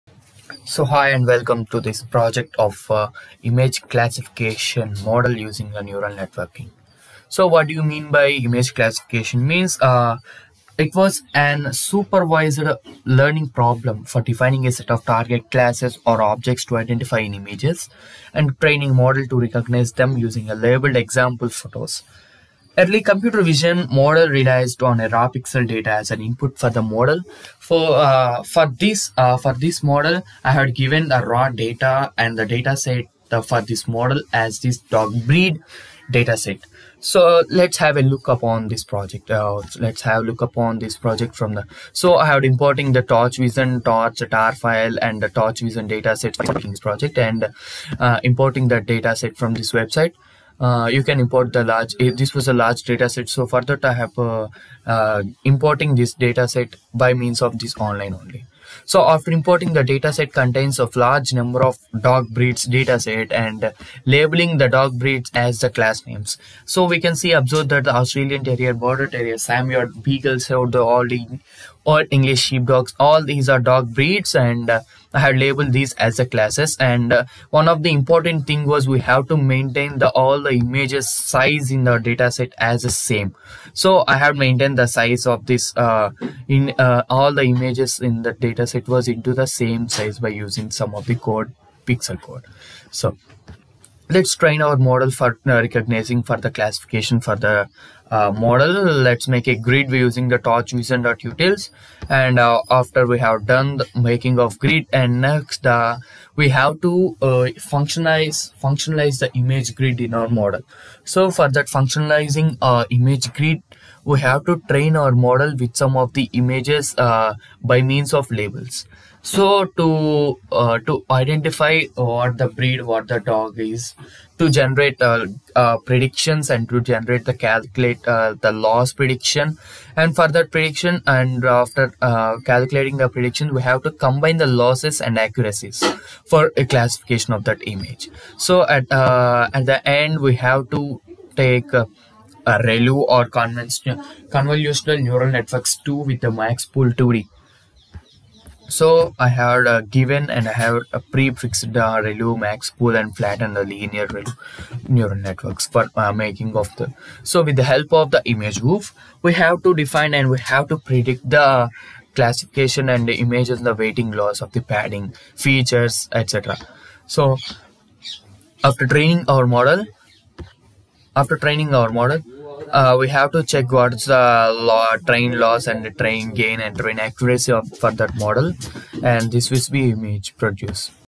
Image-Classification | RNN Deep-Learning | Hackathon Submission ( The Python Week )
The PYTHON WEEK - 2021 by Mahasena Developers Group.

Team-Name: PyTorch Barriers
Team-Members:
1.)  S.Venkata Sreeram
2.) Tejasri Naga durga Danda

The problem Image-Classification solves:
We Know as a human, we can recognise the objects easily with our Eyes and recognise with our brain. Then, How can Machines do Identify the Objects, Images, How can they Identify and classify the objects or Images. The Solutions is "Neural-Networks". Yes, It was Possible to classify the Images with the help of Neural Networks. In this project I had developed a model that can classify the images with the help of some python Libraries like torchvision, seaborn, PyTorch... classification process is to categorize all pixels in a digital image into one of several land cover classes, or "themes". This categorized data may then be used to produce thematic maps of the land cover present in an image.

Challenges we ran into:
For Classification, we need a large dataset. I was bit harder to load a larger dataset as we are having low amount of GPU. It was necessary to classify every image in the dataset. We have taken an image data set containing nearly 0.4M Images. to perform certain Operation, we have to classify every image. Sometimes Kernel was becoming "DEAD-STATE" due to shortage of GPU. this will create some illingius and might be risky one too. to overcome this, we are working on some advanced python libraries like tensorflow & keras.

Technologies we used;
PyTorch, Deep- Learning, RNN, torchvision